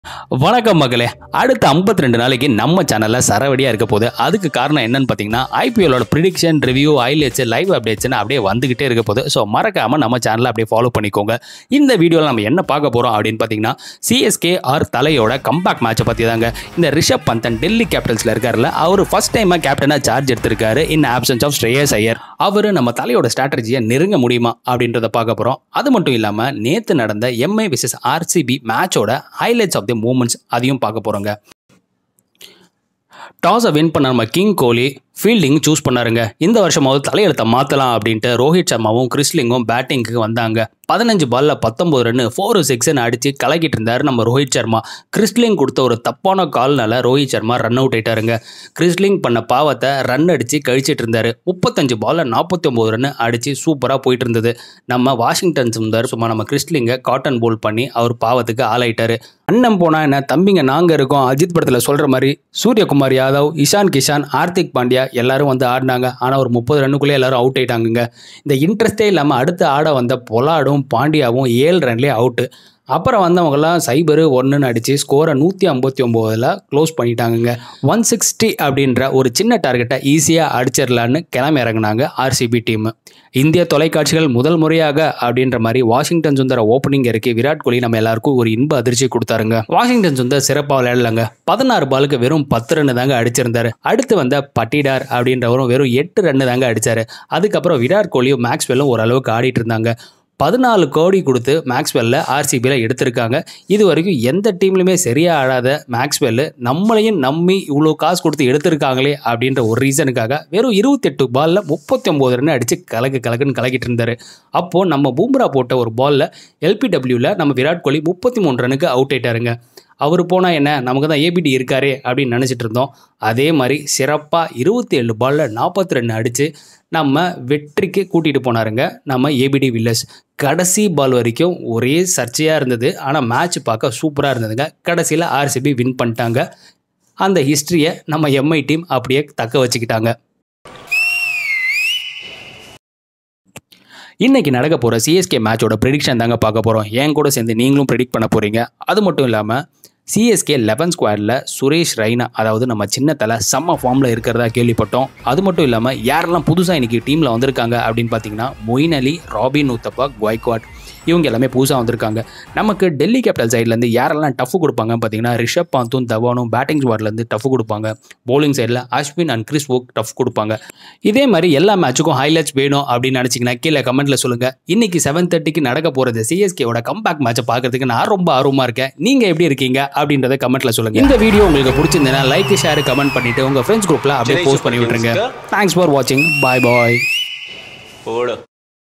CSK Comeback Match Review MI VsRCB Review
IPL 2021 - Match Review

In this video -  Review, Highlights,Prediction, Other Updates

Quick Highlighits about Match 1

Prediction about CSK Vs DC